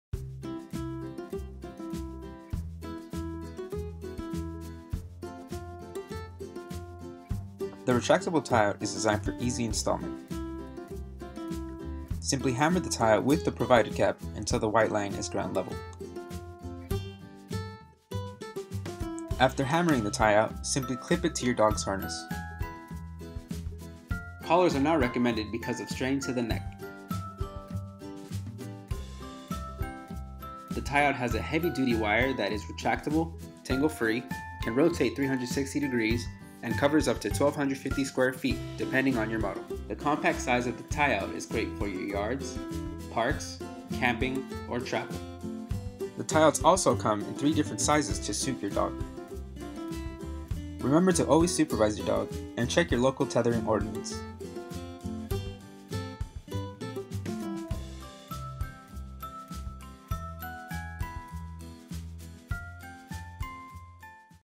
Lixit Retractable Tie Out
This video shows you how to use the Lixit Dog Tie Out and it's features.